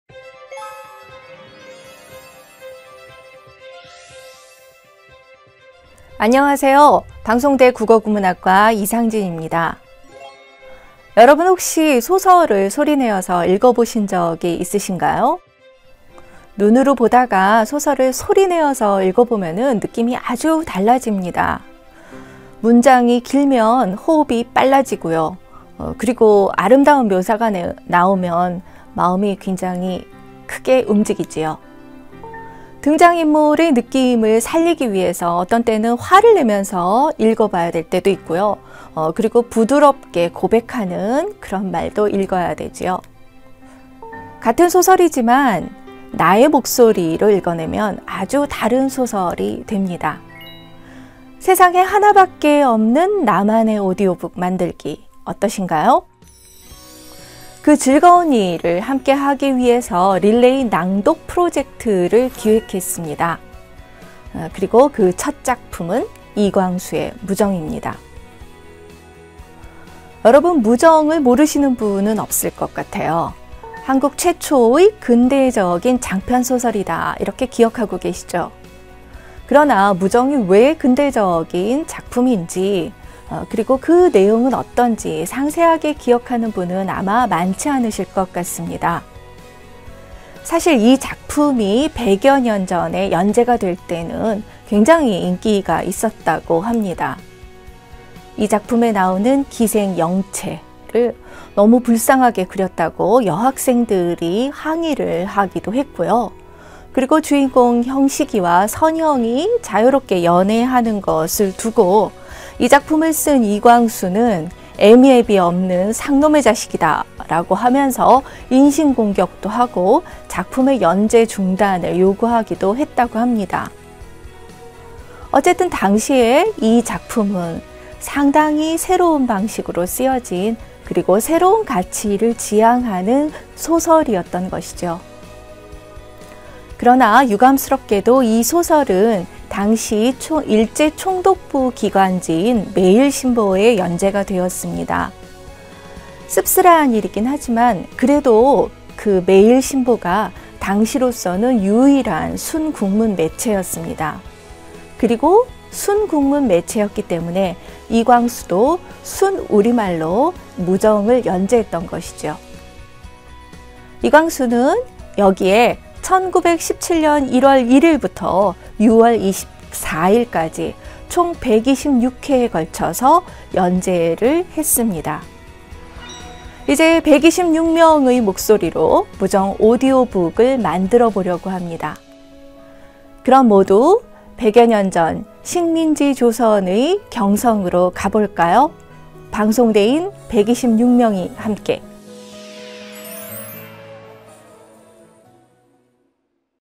[1차 모집완료] 126명 함께 하는 낭독 프로젝트 with 이상진교수 방송대
1차 모집이 완료 되었습니다.^^
지원자분의 스케줄 및 제작일정에 따라
추가적인 모집이 있을수 있으며,
2차 모집은 추후 영상을 통해 알려드리겠습니다.~
4월 중순까지는 지원자분들의 녹음 및 일정이 잡혀있습니다.
지원자 분들께는 4월 중순부터 순차적으로 연락 드릴 예정입니다.
연락이 안온다고 초조해 마시구요;; 조금만 기다려 주세요^^
코로나바이러스로 인하여 대학 방문 일정 조절하여야 하는데
쉽지가 않다고 합니다.
지원자분들의 편의를 위해 최대한 노력하는
방송대학TV가 되도록 하겠습니다.^^
감사합니다. 행복하세요~

일반인과 방송대인이 함께 만들어가는
2020 릴레이 낭독프로젝트!
나만의 오디오북도 만들고 선물도
받으세요~